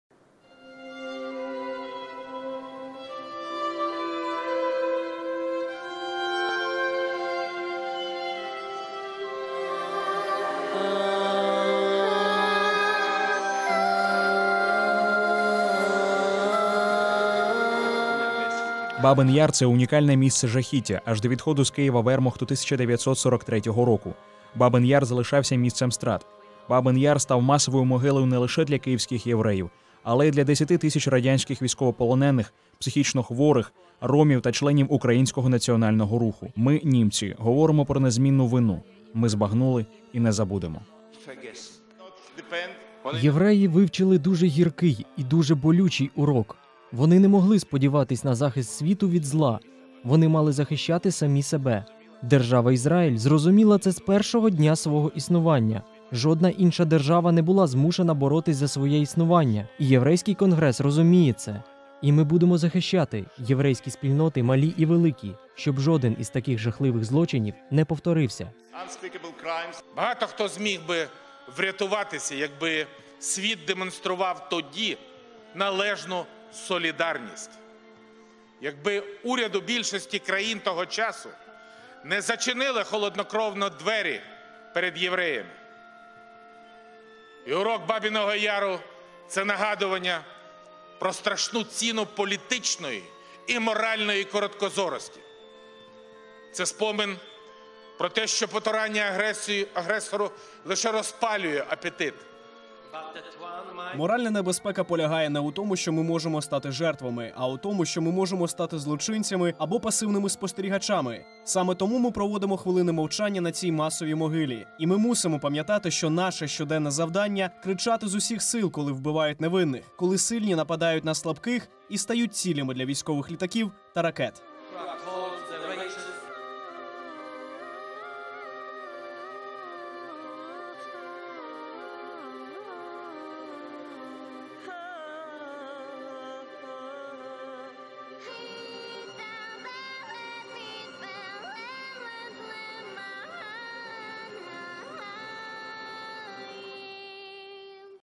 В Киеве почтили память жертв массовых расстрелов в Бабьем Яру
В Киеве в Национальном историко-мемориальном заповеднике «Бабий Яр» почтили память жертв массовых расстрелов во время Второй мировой войны в Бабьем Яру.